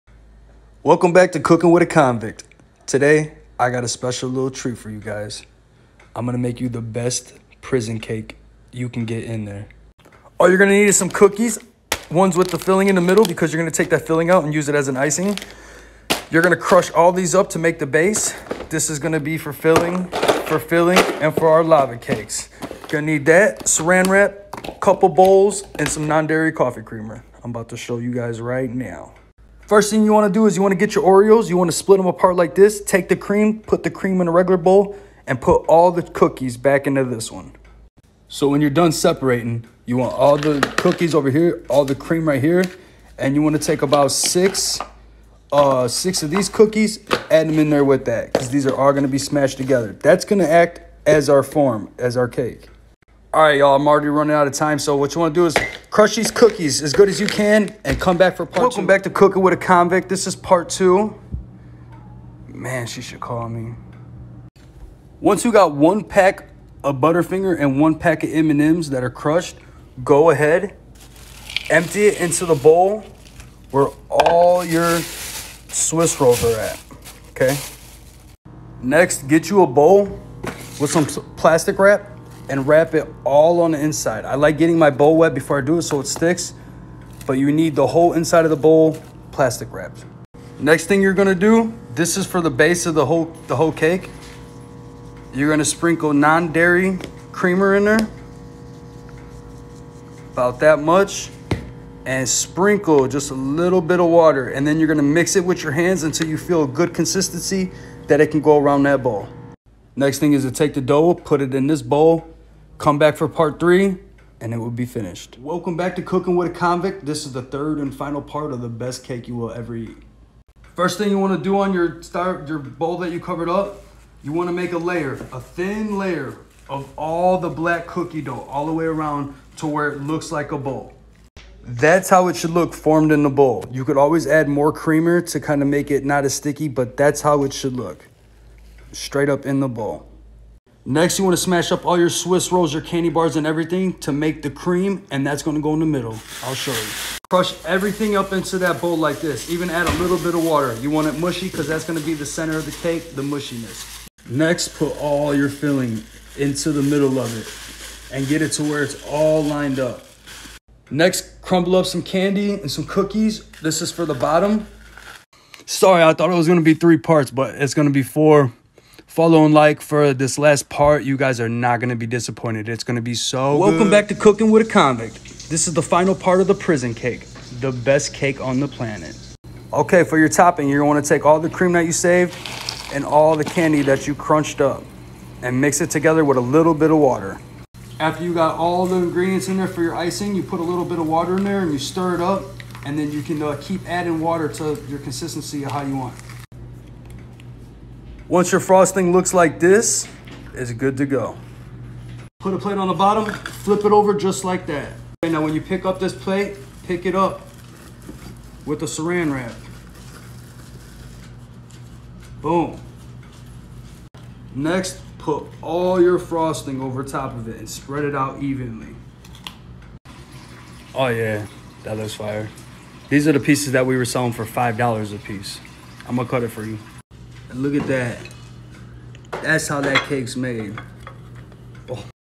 Prison cake
If you like sweets and you like chocolate then you will love to make this cake. One of the tastiest sweetest cakes you are roommate